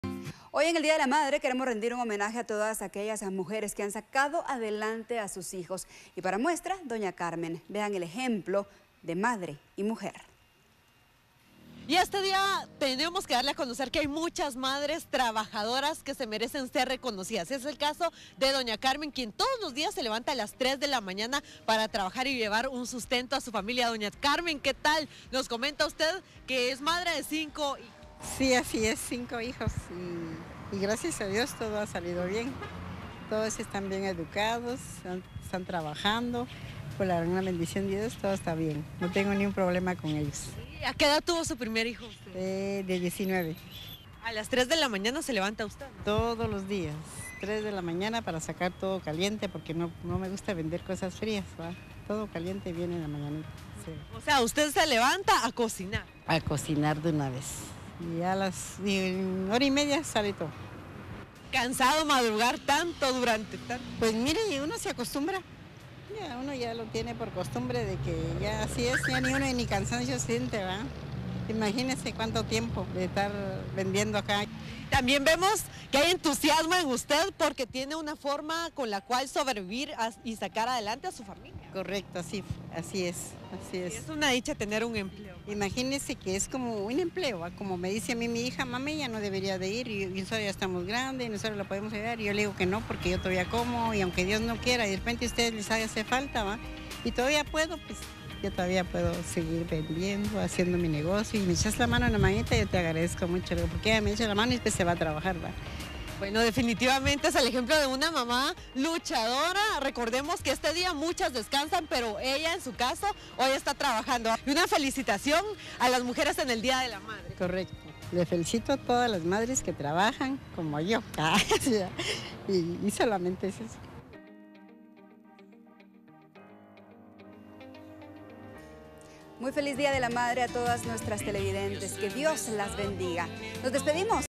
Doña Carmen ejemplo de mujer esforzada y trabajadora
Conmemoramos a las madres es su día

Doña Carmen es madre de cinco hijos a diario madruga para preparar sus panes y salir a venderlos para llevar el sustento a su familia.